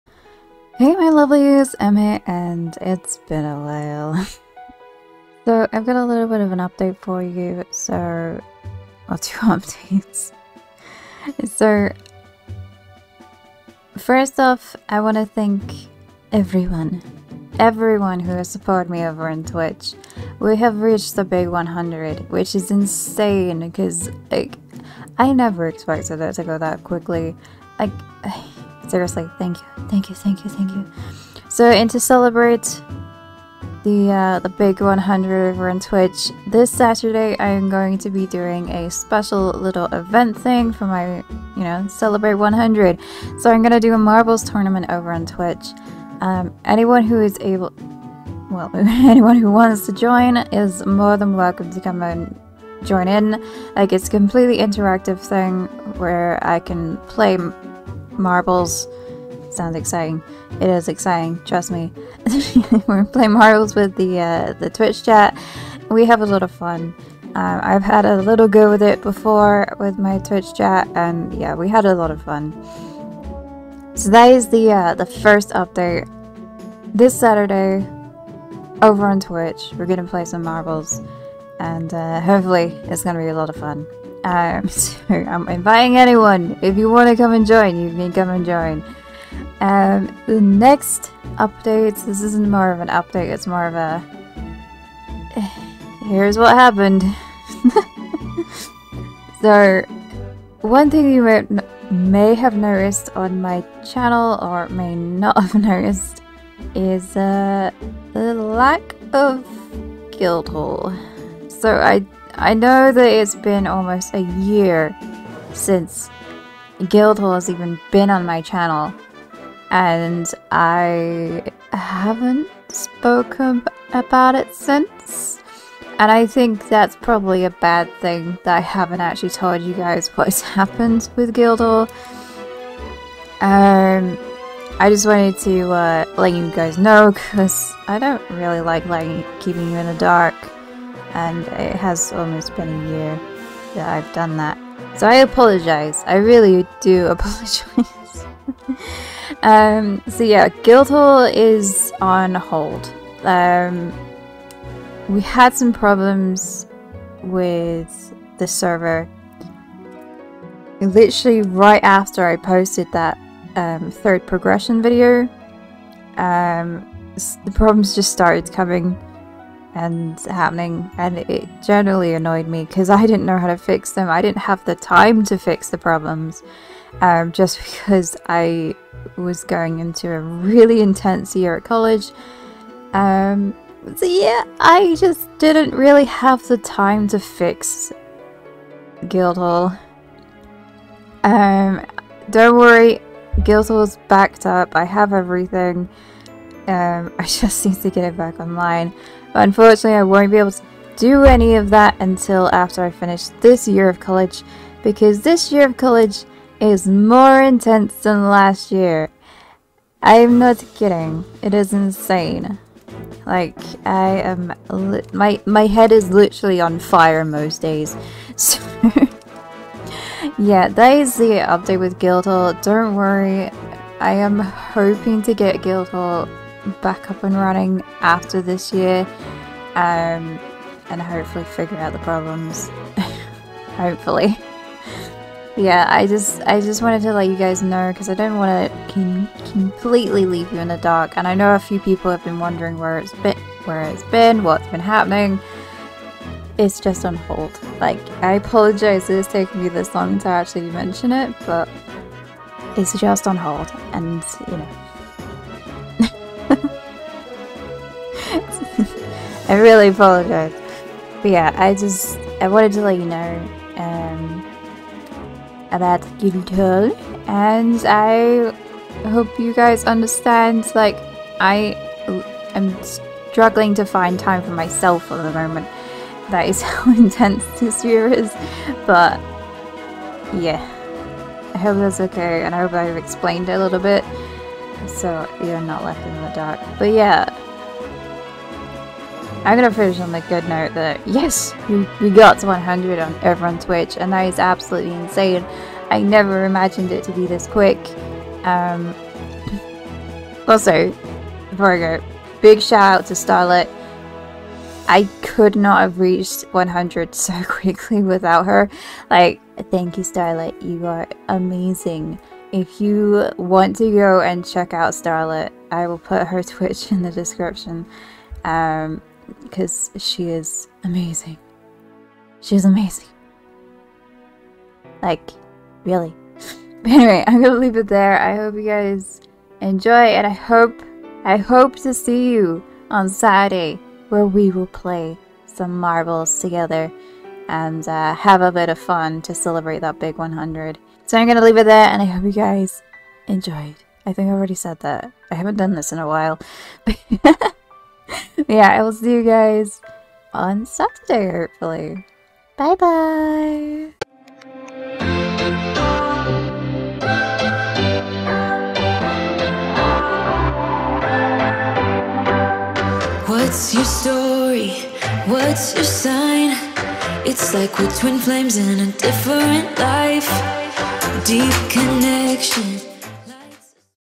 Little Update | Twitch + GuildHall chat
Little update for you today as I thank you those that have supported me over on Twitch as we've just reached 100 followers. I'll be doing a little something this Saturday to celebrate.
I also have a little chat about the current status of GuildHall as its almost been a year since I've spoken about it (oops).